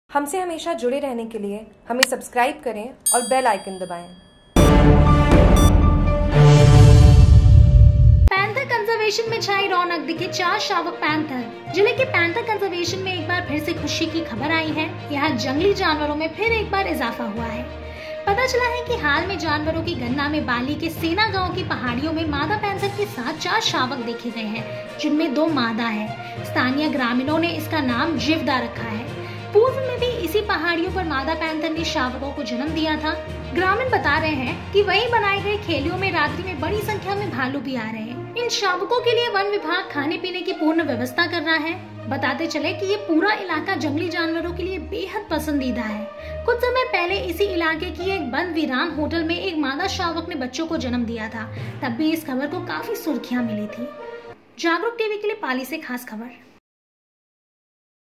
Jagruk TV पाली : पेंथर कंजेर्वशन में छाई रौनक, दिखे चार शावक पेंथर
पाली । जिले के पेंथर कंवज्रेशन में एक बार फिर से खुसी की खबर आई है। यहां जंगली जानवरो में फिर एक बार इजाफा हुआ है।

पता चला है कि हाल में जानवरों की गणना में बाली के सेना गांव की पहाड़ियों में मादा पेंथर के साथ चार शावक देखे गए है। जिनमे दो मादा हैं।

स्थानीय ग्रामीणों ने इसका नाम जिवदा रखा है।पूर्व में भी इसी पहाड़ियों पर मादा पेंथर ने शावको को जन्म दिया था।ग्रामीण बता रहे हैं कि वही बनाई गई खेलिओ में रात्रि में बड़ी सँख्या में भालू भी आ रहे।

इन शावको के लिए वन विभाग खाने पीने की पूर्ण व्यवस्था कर रहा है। बताते चले कि यह पूरा इलाका जंगली जानवरों के लिए बेहद पसंदीदा है। कुछ समय पहले इसी इलाके की एक बंद वीरान होटल में एक मादा शावक ने बच्चों को जन्म दिया था। तब भी इस खबर को काफी सुर्खियां मिली थी

Jagruk TV